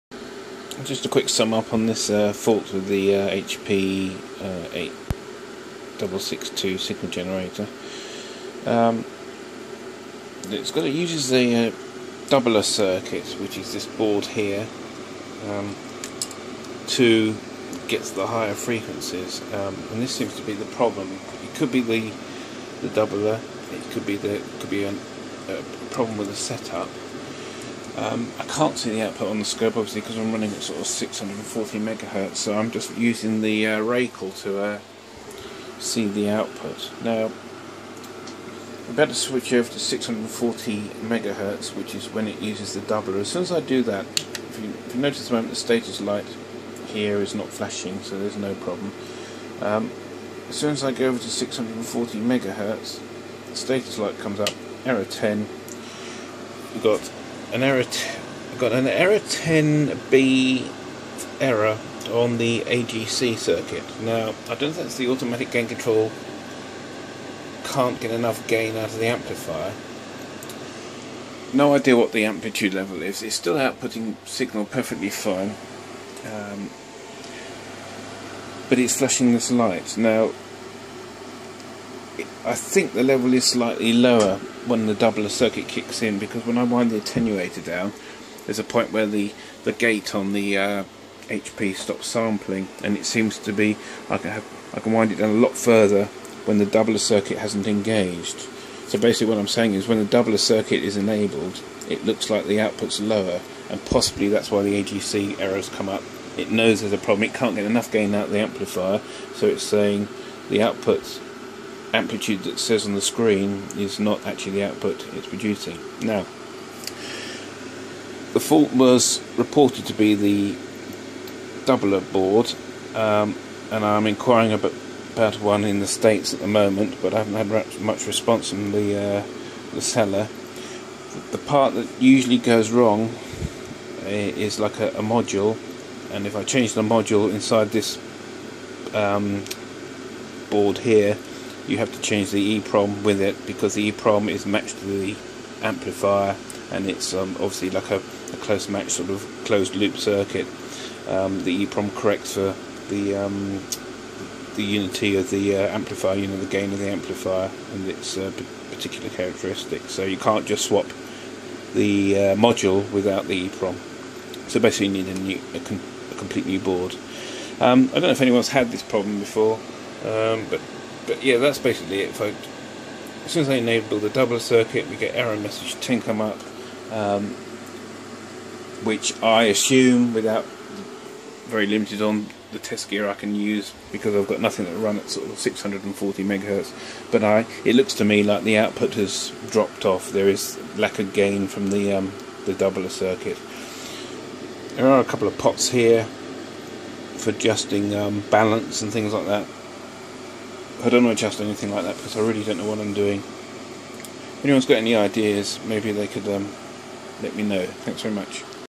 HP 8662A pt 3 fault manifestation and surmise
Update 29-01-14 another doubler board is on its way from the States. Hopfully this will rectify the this fault.